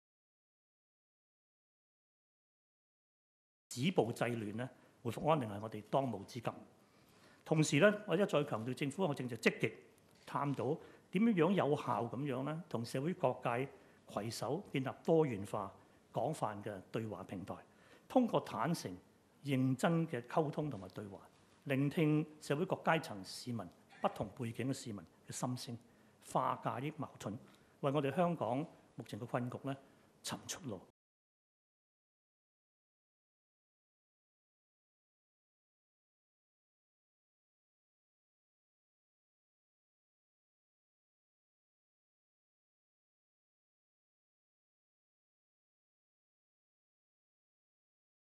It's Imperative to Stop Violence, End Chaos: Chief Secretary for Administration
Chief Secretary for Administration of the Hong Kong Special Administration Region (HKSAR) Government Matthew Cheung Kin-chung called for stopping violence and ending chaos as a top priority for the region at an inter-departmental press conference on Monday.